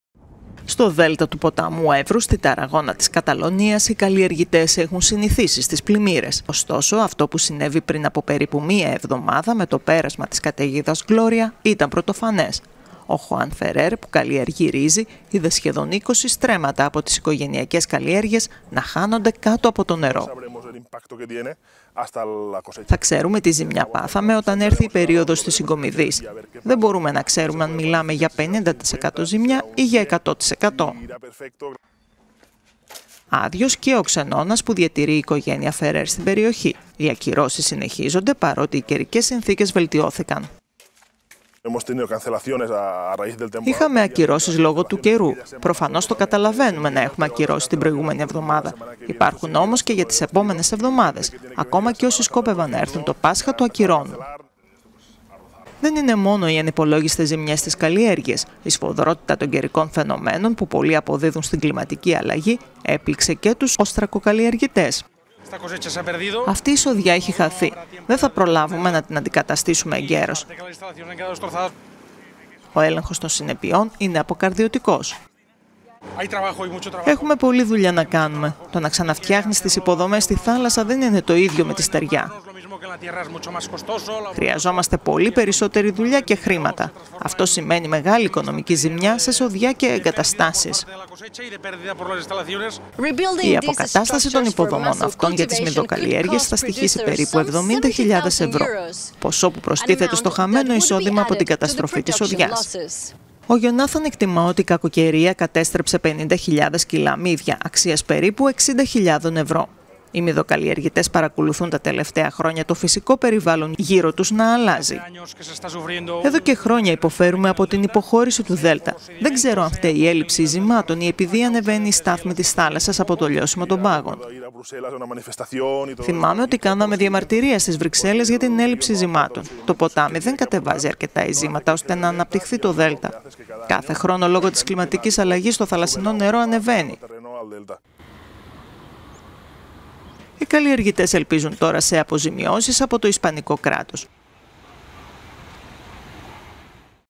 Η κλιματική αλλαγή προκαλεί τεράστιες ζημιές στους Ευρωπαίους αγρότες
Τελευταίο παράδειγμα οι καλλιεργητές στο Δέλτα του Έβρου, στην Καταλονία…